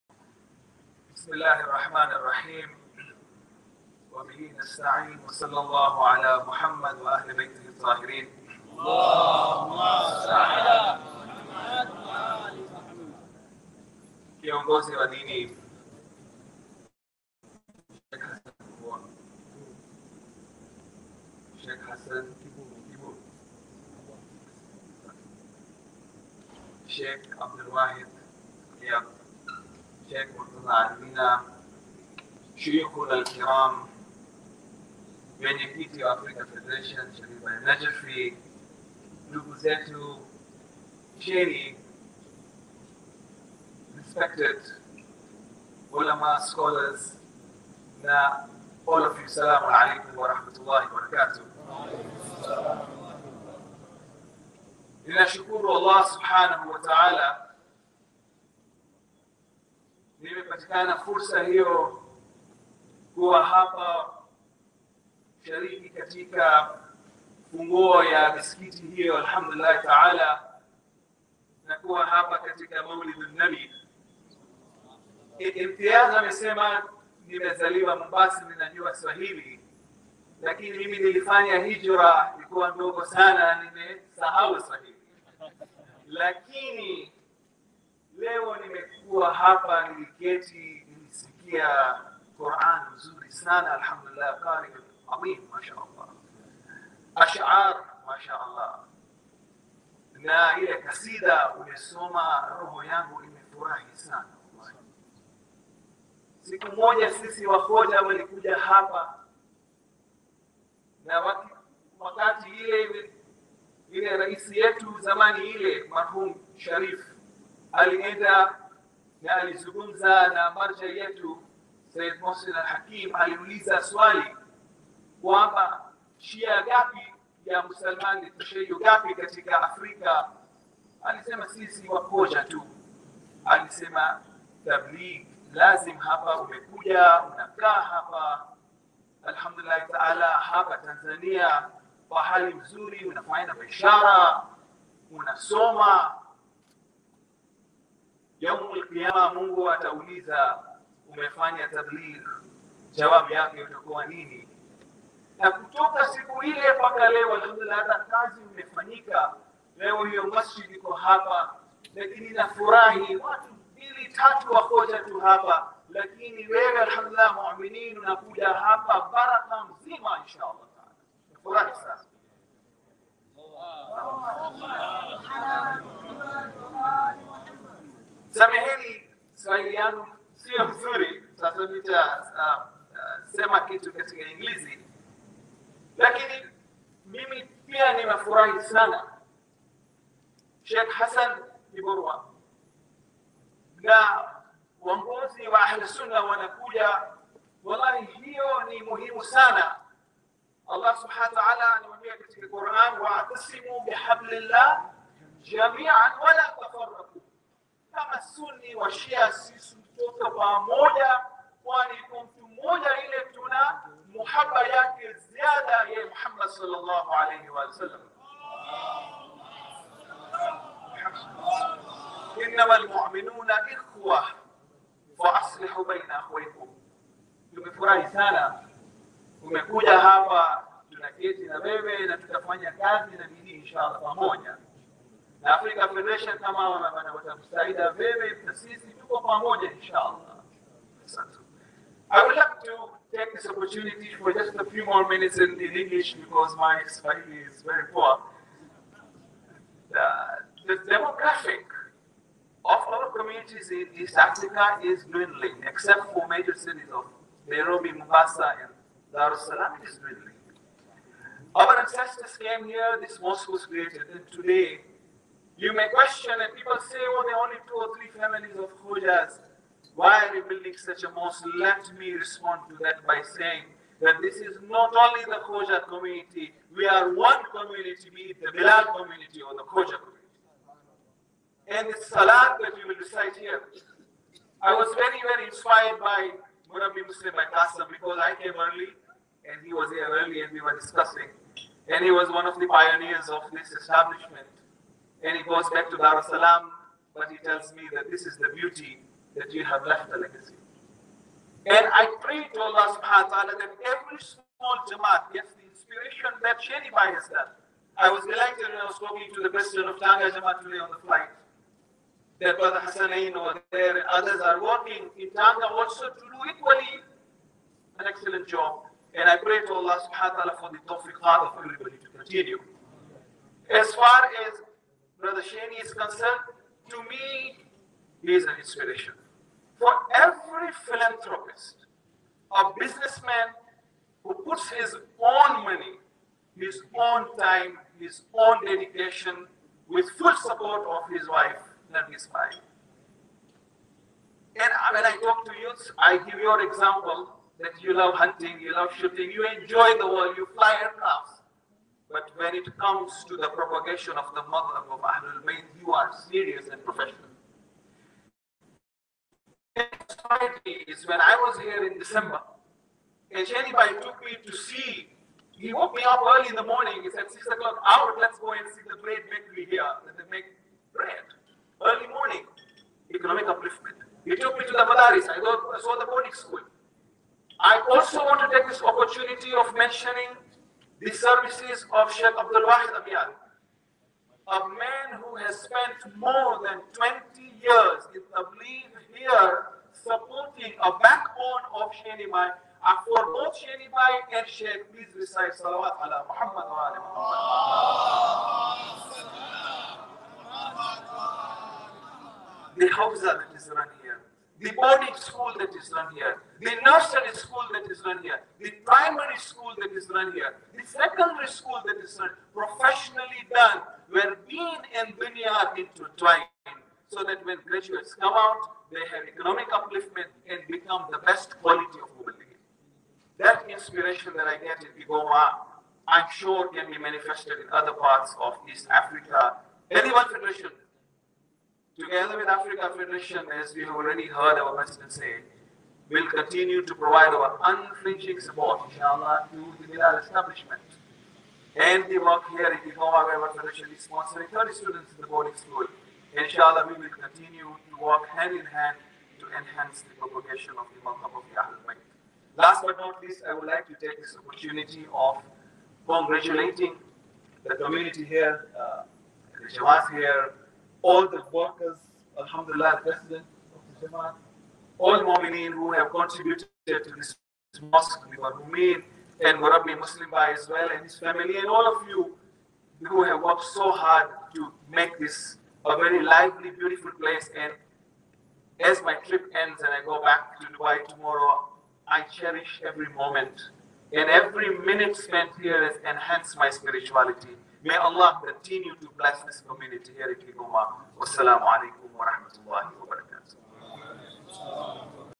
President's address at the opening of KSIJ Mosque in Kigoma
Watch the address of The World Federation, Safderbhai Jaffer's, address at the opening of the Khoja Mosque in Kigoma where he travelled together with the President of KSIJ Dar, Alhaj Mohamedrazabhai Dewji and Head of Islamic Education, Shaykh Murtadha Alidina.

Read more about his trip to Tanzania at: